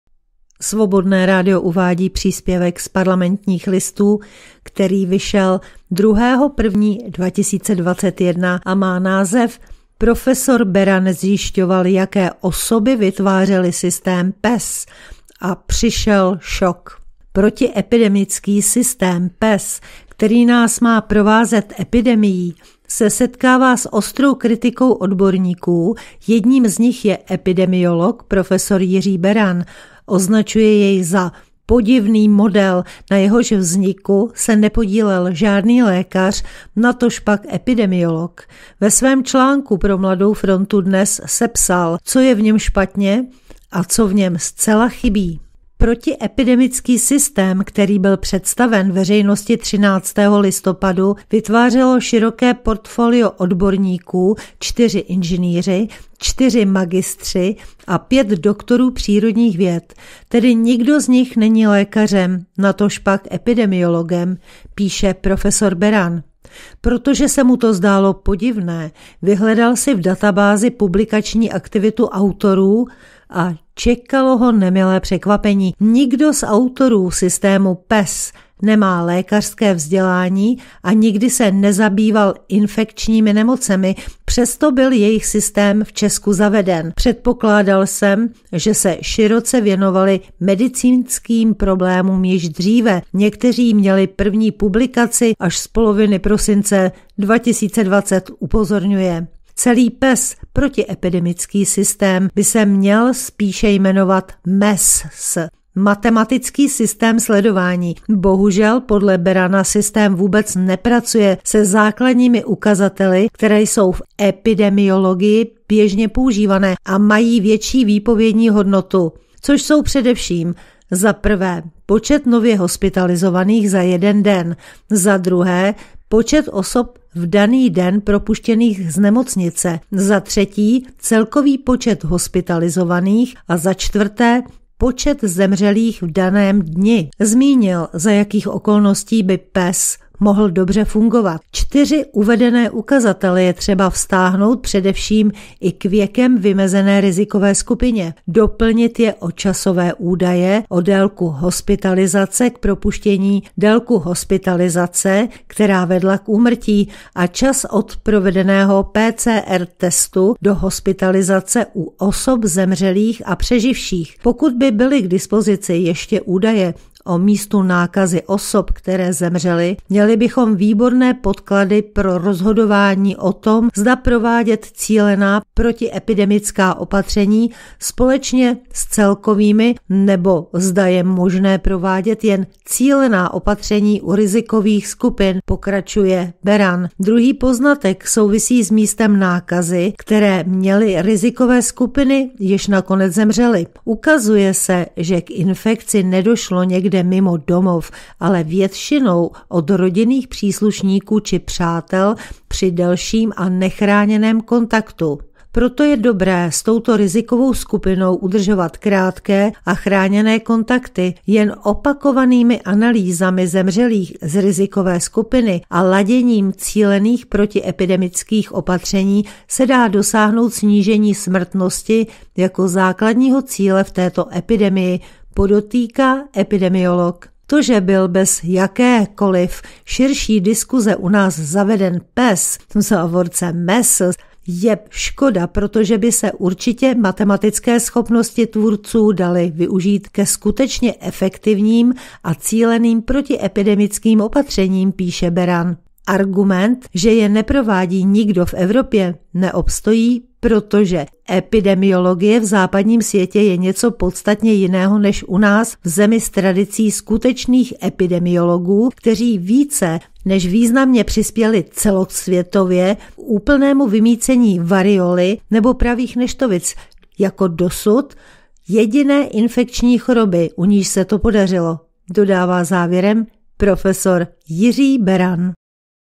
2021-01-03-Parlamentní listy-Profesor Beran zjišťoval, jaké osoby vytvářely systém PES.A přišel šok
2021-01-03 - Studio Kladno - Parlamentní listy - Profesor Beran zjišťoval, jaké osoby vytvářely systém PES. A přišel šok

Podpořte prosím finančně chod Svobodného rádia:
Číslo účtu: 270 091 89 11/2010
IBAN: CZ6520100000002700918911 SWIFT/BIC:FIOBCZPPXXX
Kompletní archiv Svobodného rádia: